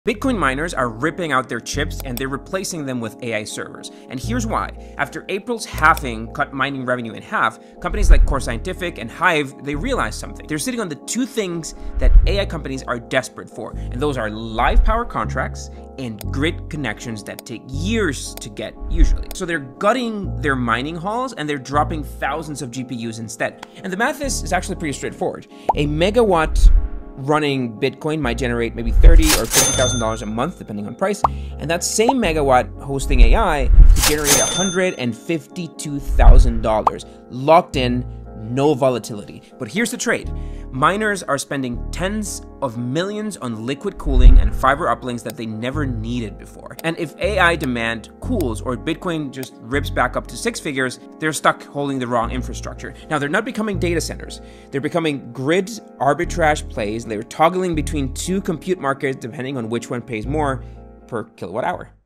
Bitcoin miners found a better customer than Bitcoin
Why Bitcoin miners are pivoting into AI infrastructure.

Post halving economics have pushed Bitcoin miners to rethink their business models.
With access to scarce power contracts and grid connections, many are repurposing facilities to host AI workloads instead of mining crypto.

While AI hosting offers stable, contract based revenue, it requires heavy investment in cooling and networking infrastructure and introduces new risk if market conditions shift.

The result is a new hybrid model where operators arbitrage between compute markets based on energy profitability.

👉 Visit our YouTube channel for more content.